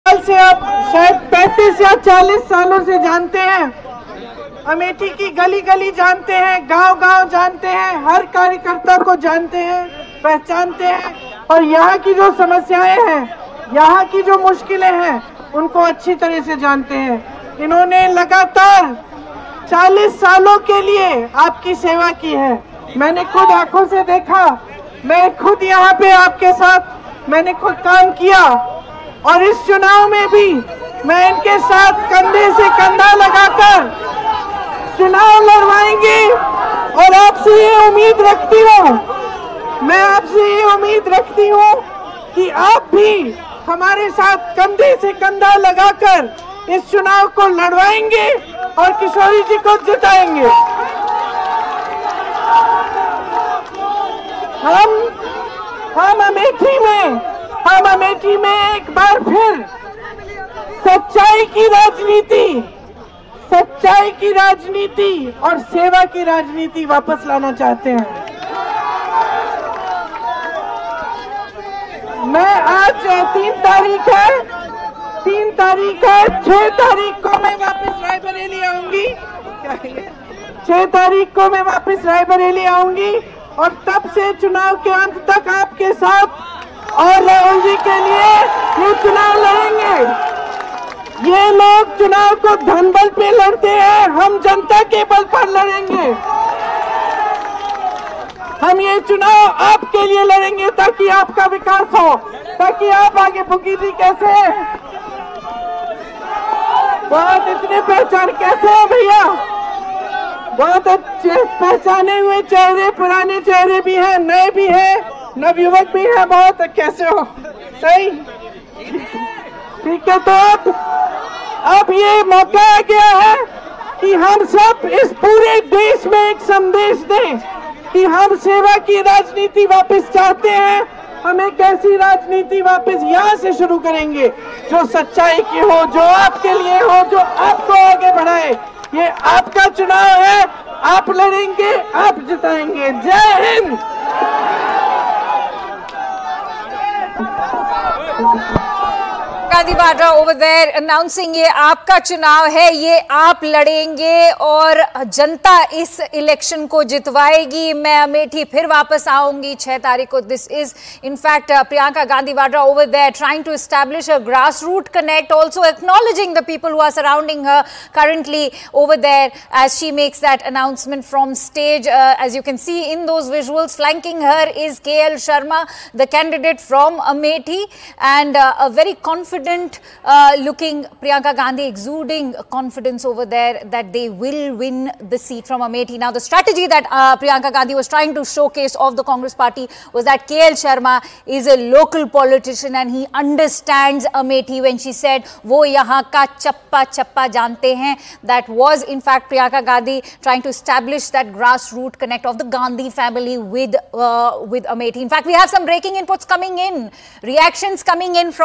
Priyanka Gandhi Vadra Addresses Rally In Amethi | KL Sharma | Congress News | Lok Sabha Election
UttarPradesh: Congress General Secretary Priyanka Gandhi Vadra addresses rally in Amethi

Congress leader Rahul Gandhi will contest from Uttar Pradesh’s Raebareli, a seat represented by his mother Sonia Gandhi for four terms until she was elected to Rajya Sabha this year. The party’s decision came on the last day for nomination filing by candidates for the prestigious seat.

In its eleventh-hour decision, the Congress fielded Kishori Lal Sharma against Union Minister Smriti Irani in Amethi, where the Gandhi scion suffered defeat in 2019 elections.

n18oc_india